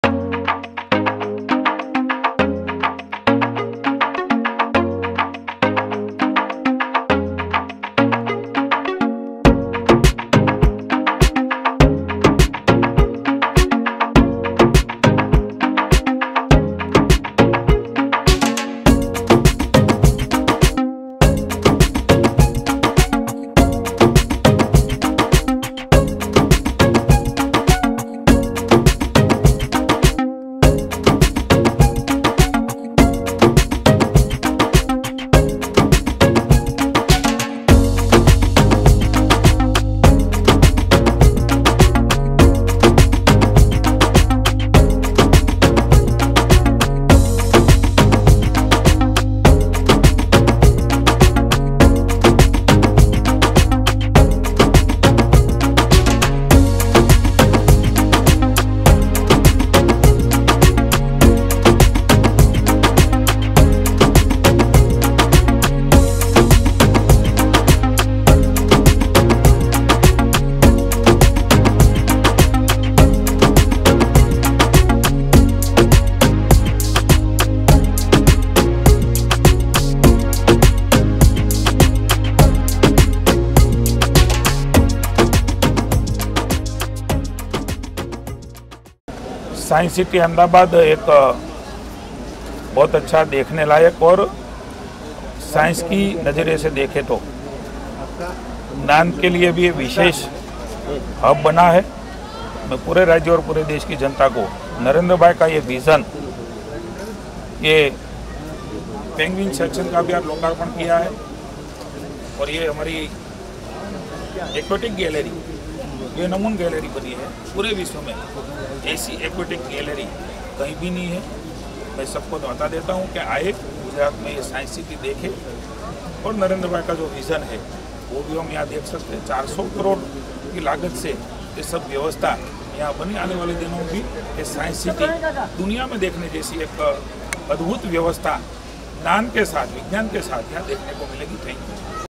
Penguins in Gujarat ; Science City Ahmedabad brings them from South Africa for its Aquatic Gallery
Ahmedabad: Gujarat Science City based aquarium has got five penguins. Science & Technology minister Jitubhai Vaghani today inaugurated the facility which would be extremely attractive for visitors, especially the children as the penguins are found far away from Indian subcontinent, in coldest areas of the world. These particular penguins are African ones from coastal South Africa. Already busy with number of visitors from all over Gujarat and India, Science City is going to be further busier with arrival of penguins. DeshGujarat